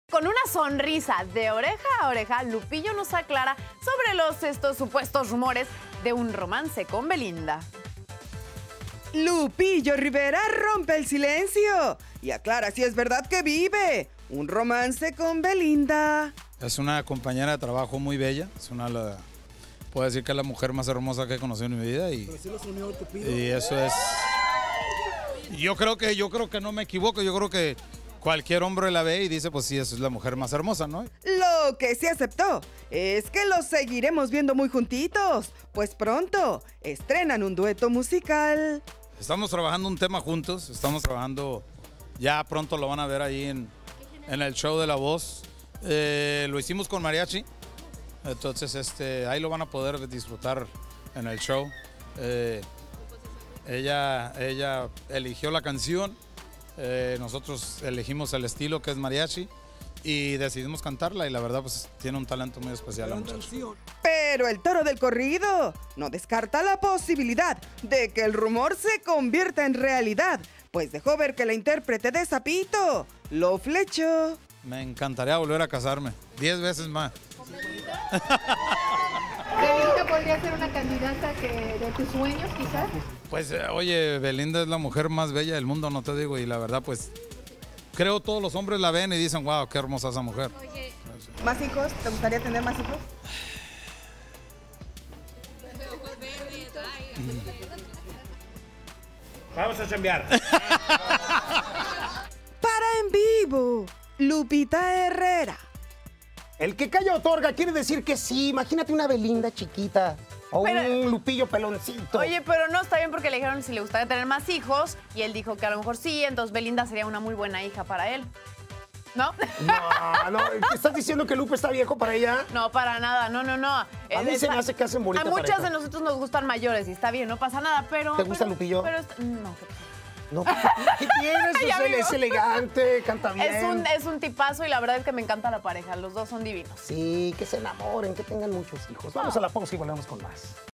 Lupillo Rivera nos aclara los rumores sobre su supuesto romance con Belinda-en vivo
Lupillo Rivera nos aclara los rumores  sobre su supuesto romance con Belinda